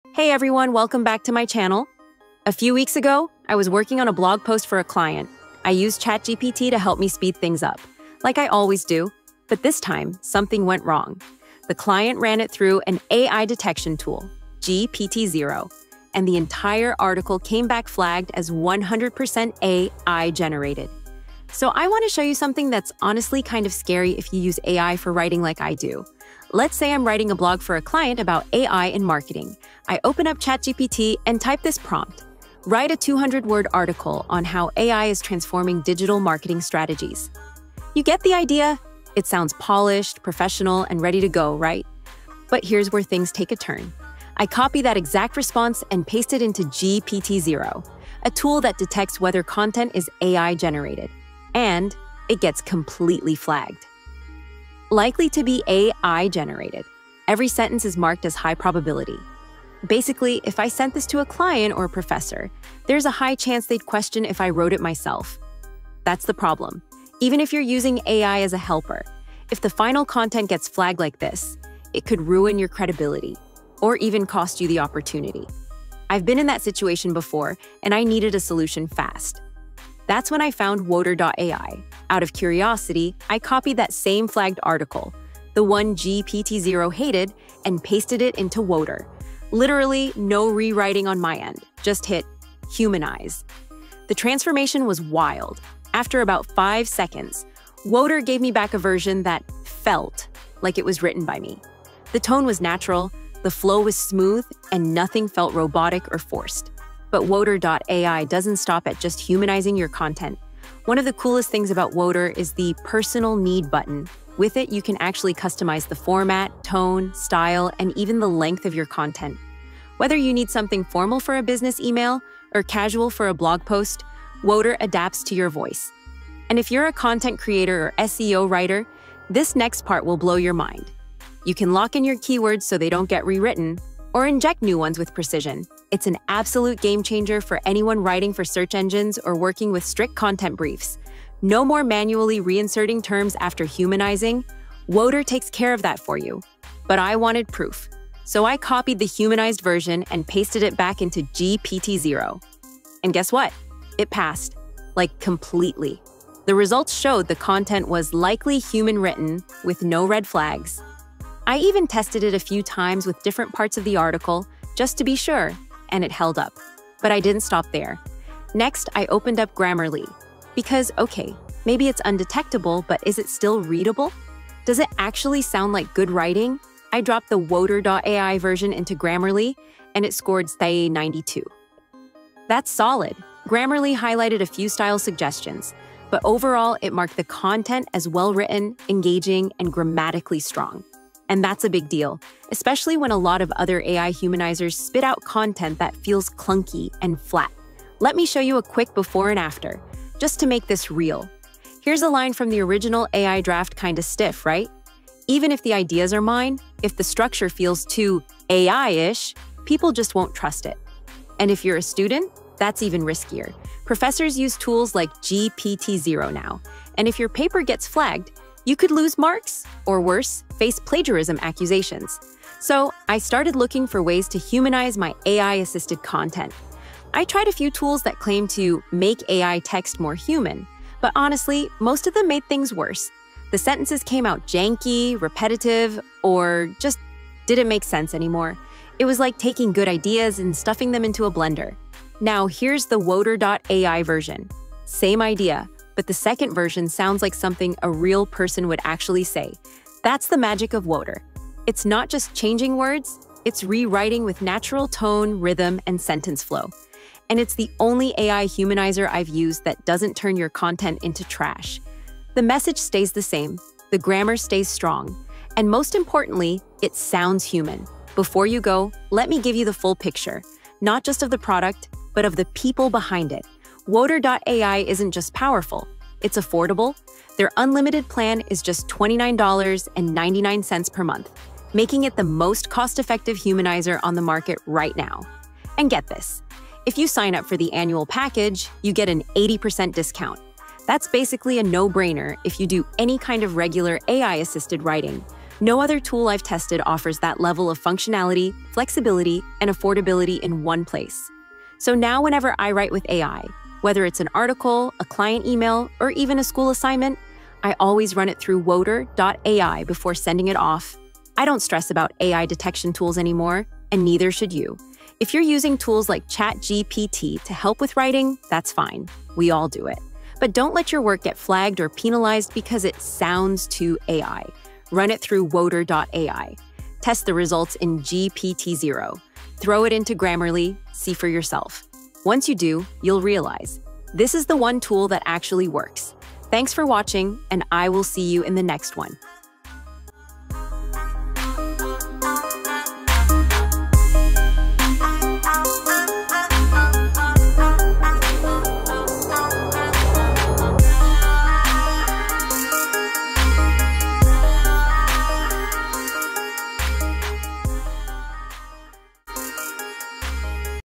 How to Make AI Text Look Human 100% Undetectable with Woter.ai Humanize ai content
Want to make your AI-generated content look 100% human and bypass all major AI detectors like GPTZero, Turnitin, and Originality.ai?

In this video, we’ll show you how to use Woter.ai, a powerful AI humanizer tool that rewrites your AI text to make it undetectable — while keeping it natural, high quality, and plagiarism-free.

🔍 What You’ll Learn:

How AI detectors work and what they flag

How to use Woter.ai to humanize any text

Live demo: Turning flagged content into clean, human-sounding writing

Pros, cons, and safety tips when using AI humanizers

Best use cases: content writing, blogging, academic work, and more

🚀 Whether you're a student, freelancer, or content creator, this tool can help you stay ahead in a world full of detection tools.